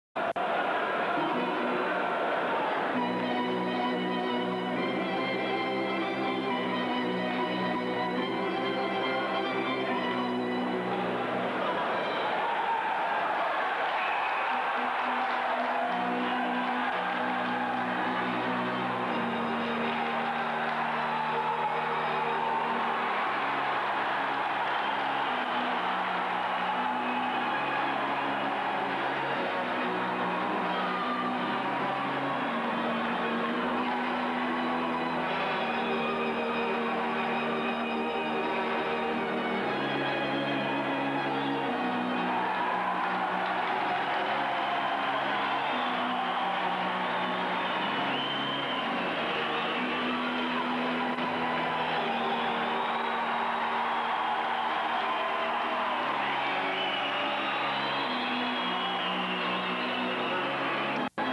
John Kovacs vs Marc Rodgers LHJMQ 2 02 92
Laval Titan at Verdun Collège Français LHJMQ QMJHL 1992